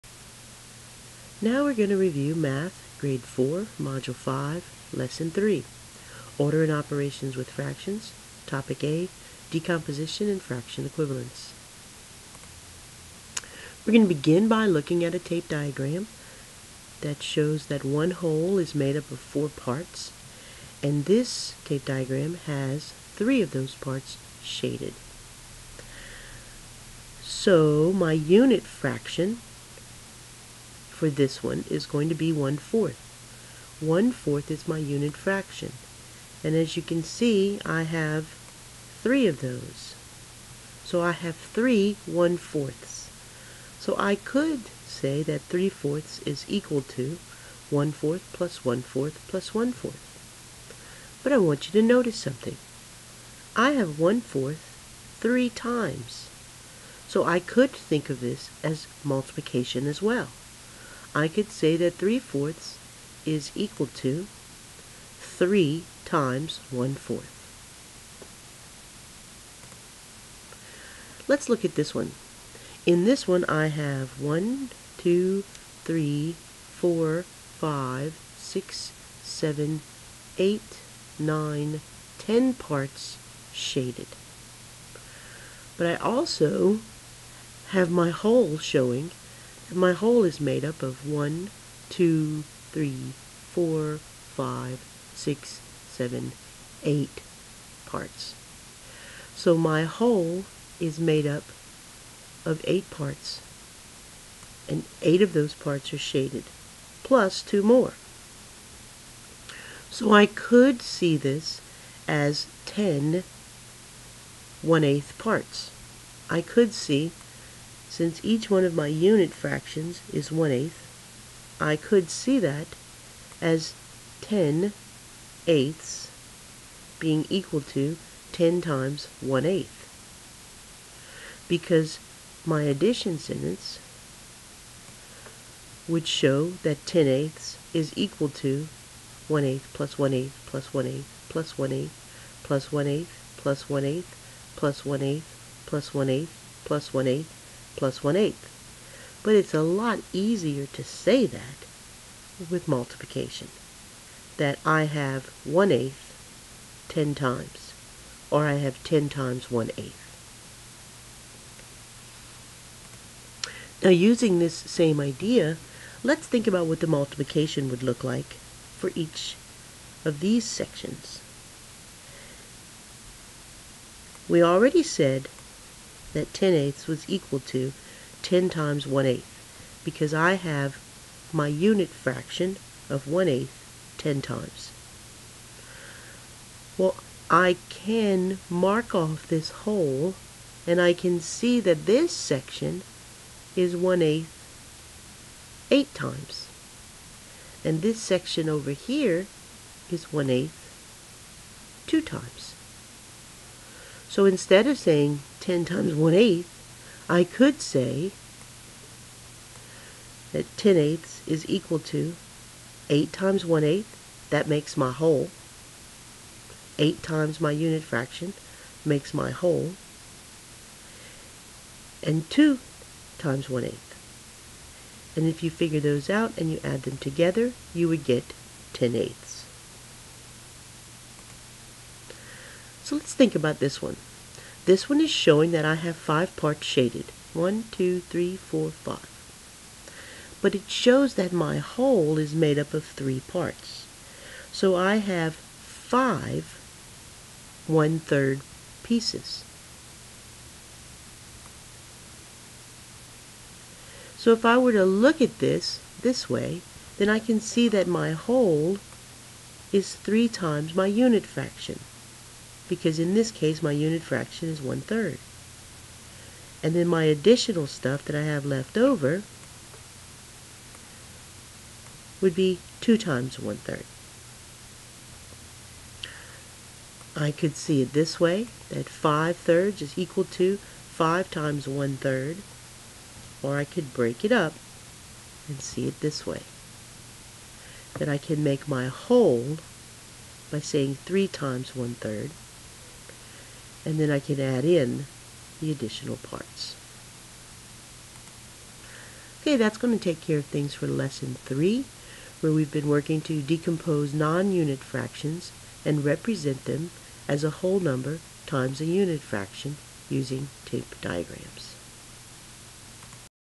Grade 4 Mod 5 Lesson 3
Teacher explains math lesson for 4th grade module 5 lesson 3.